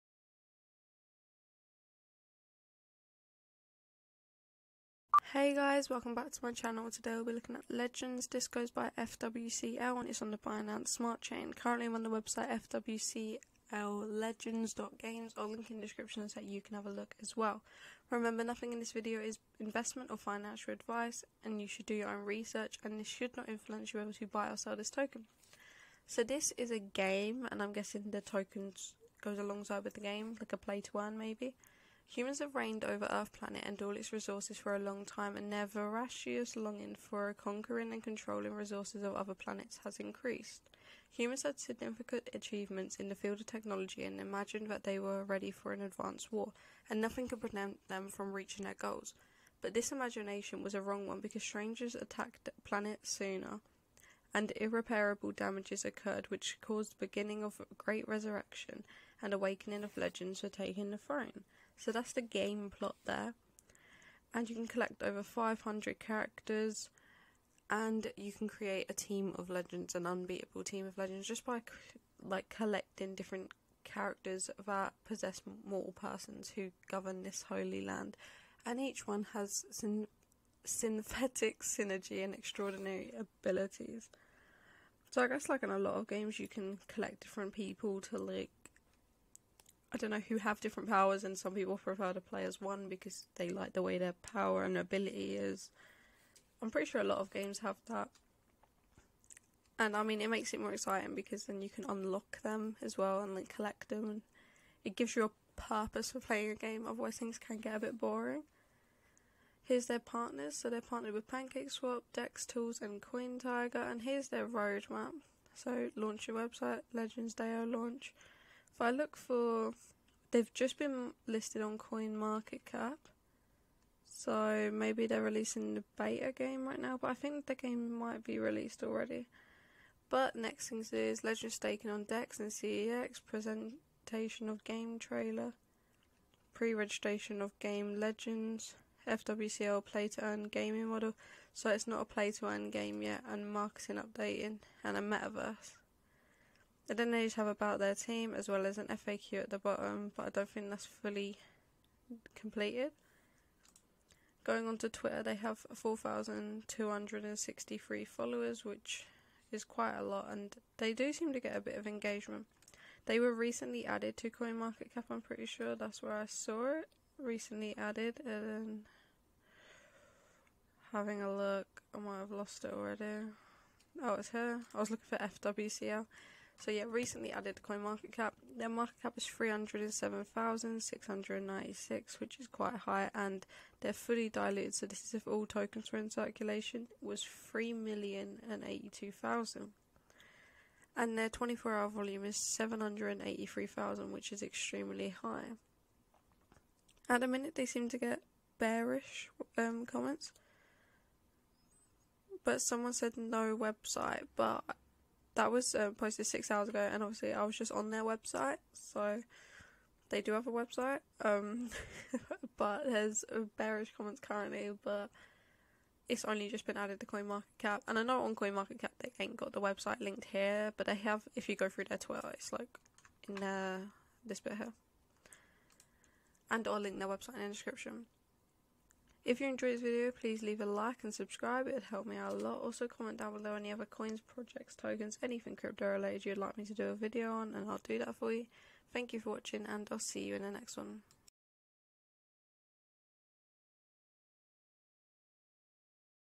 LEGENDS GAME - $FWCL - BINANCE SMART CHAIN - BSC
In this video i look at Legends and their website!

Thankyou for watching and reading!!